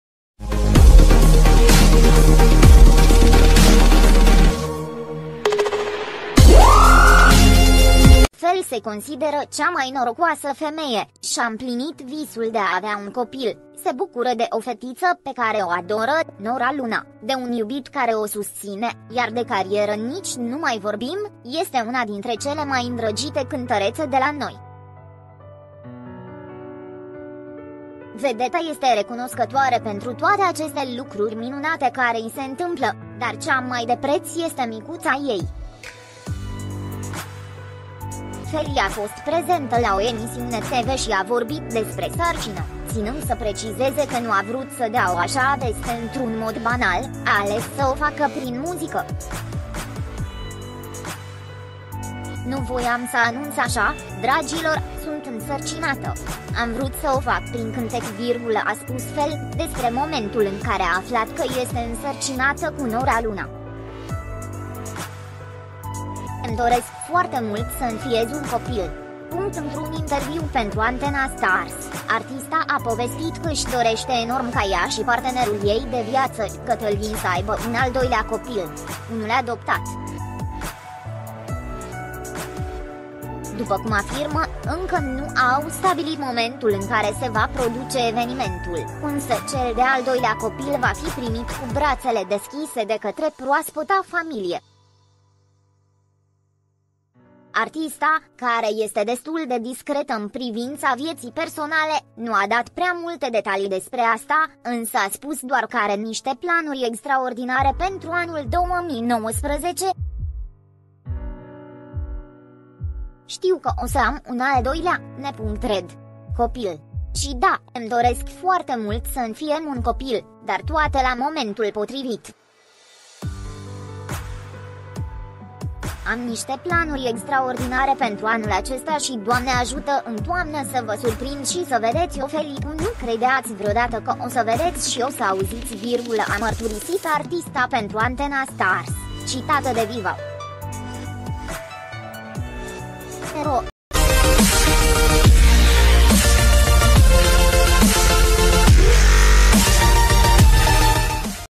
FELI A VORBIT DESPRE SARCINĂ. A FĂCUT ANUNŢUL ÎN DIRECT: „DRAGILOR, SUNT ÎNSĂRCINATĂ”
Feli se consideră cea mai norocoasă femeie, şi-a împlinit visul de a avea un copil, se bucură de o fetiţă pe care o adoră, Nora Luna, de un iubit care o susţine, iar de carieră nici nu mai vorbim, este una dintre cele mai îndrăgite cântăreţe de la noi.

Vedeta este recunoscătoare pentru toate aceste lucruri minunate care i se întâmplă, dar cea mai de preţ este micuţa ei. Feli a fost prezentă la o emisiune TV şi a vorbit despre sarcină, ţinând să precizeze că nu a vrut să dea o aşa veste într-un mod banal, a ales să o facă prin muzică. „Nu voiam să anunţ aşa, dragilor, sunt însărcinată. Am vrut să o fac prin cântec”, a spus Feli, despre momentul în care a aflat că este însărcinată cu Nora Luna.
”Îmi doresc foarte mult să înfiez un copil!”
Într-un interviu pentru Antena Stars, artista a povestit că își dorește enorm ca ea și partenerul ei de viață, Cătălin să aibă un al doilea copil… Unul adoptat. După cum afirmă, încă nu au stabilit momentul în care se va produce evenimentul, însă cel de-al doilea copil va fi primit cu brațele deschise de către proaspăta familie. Artista, care este destul de discretă în privința vieții personale, nu a dat prea multe detalii despre asta, însă a spus doar că are niște planuri extraordinare pentru anul 2019.

„Ştiu că o să am un al doilea (n.red. copil)! Şi da, îmi doresc foarte mult să înfiem un copil, dar toate la momentul potrivit. Am nişte planuri extraordinare pentru anul acesta şi Doamne ajută în toamnă să vă surprind şi să vedeţi o Feli cum nu credeaţi vreodată că o să vedeţi şi o să auziţi”,a mărturisit artista pentru Antena Stars, citată de viva.ro.